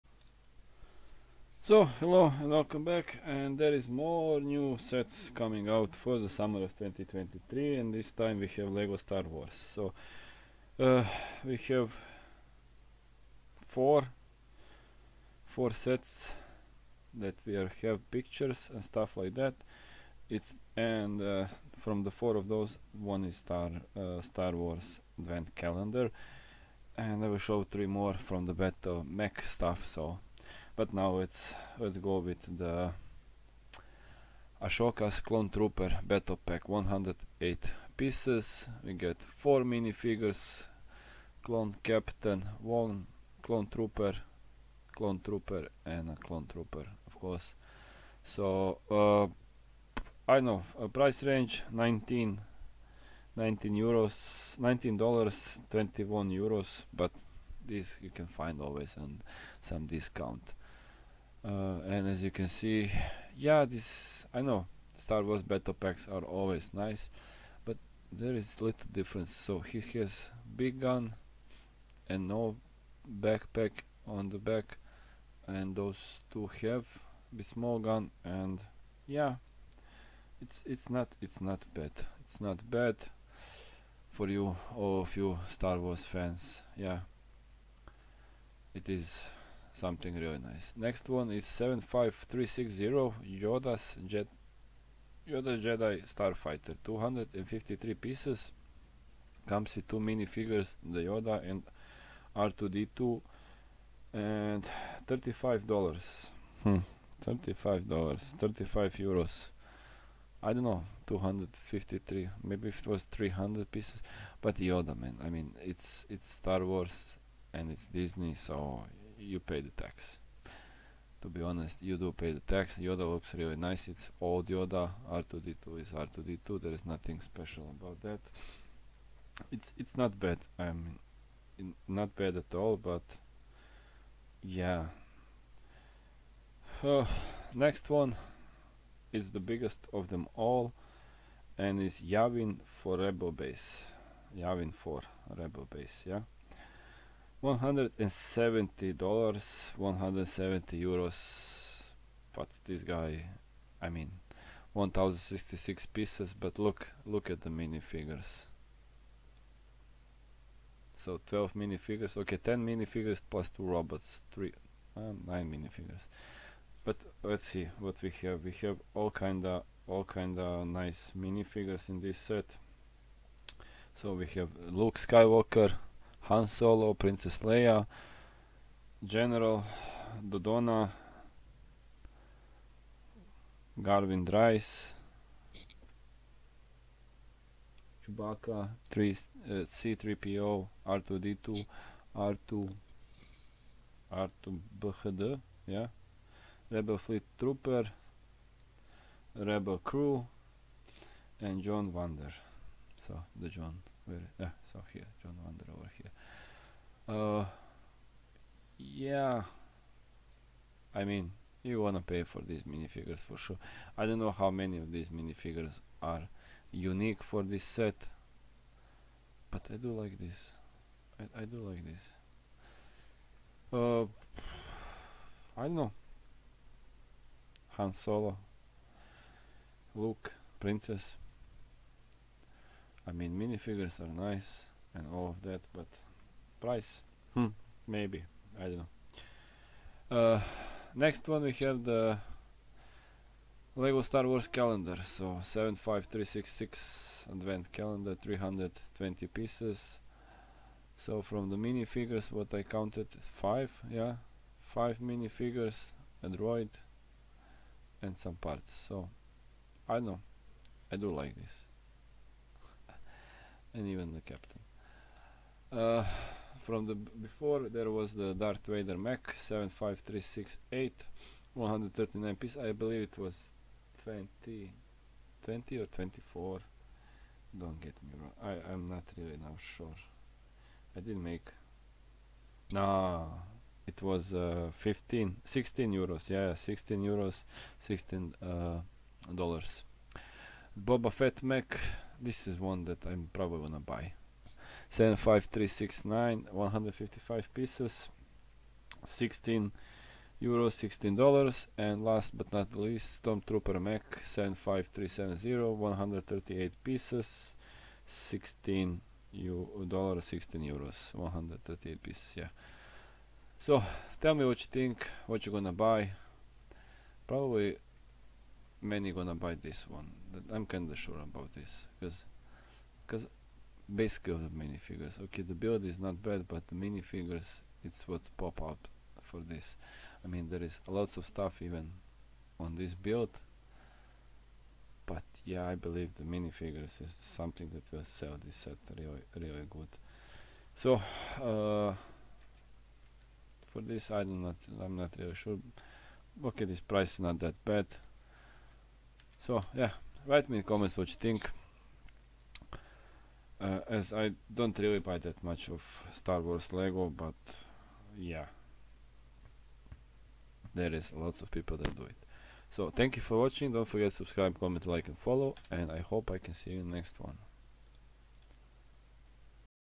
SUMMER 2023 LEGO STAR WARS SETS REVEALED
LEGO have revealed four new Summer 2023 LEGO Star Wars sets, which are set to be released on 1 August 2023, with the exception of 75366 Star Wars Advent Calendar, which will be released in September.

The small wave includes the highly anticipated clone army builder, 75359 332nd Ahsoka’s Clone Trooper Battle Pack, and 75365 Yavin 4 Rebel Base, which includes a whopping 10 LEGO minifigures and 2 Astromech droids!

#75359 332nd Ahsoka’s Clone Trooper Battle Pack – US$19.99 / AU$TBD / £18.99 / €20.99 / CAD$24.99
#75360 Yoda’s Jedi Starfighter – US$34.99 / AU$TBD / £29.99 / €34.99 / CAD$44.99
#75365 Yavin 4 Rebel Base – US$169.99 / AU$TBD / £149.99 / €169.99 / CAD$219.99
#75366 Star Wars Advent Calendar – US$44.99 / AU$TBD / £29.99 / €37.99 / CAD$59.99
#75368 Darth Vader Mech – US$15.99 / AU$23.99 / £12.99 / €15,99 / CAD$19.99
#75369 Boba Fett Mech – US$15.99 / AU$23.99 / £12.99 / €15,99 / CAD$19.99
#75370 Stormtrooper Mech – US$15.99 / AU$23.99 / £12.99 / €15,99 / CAD$19.99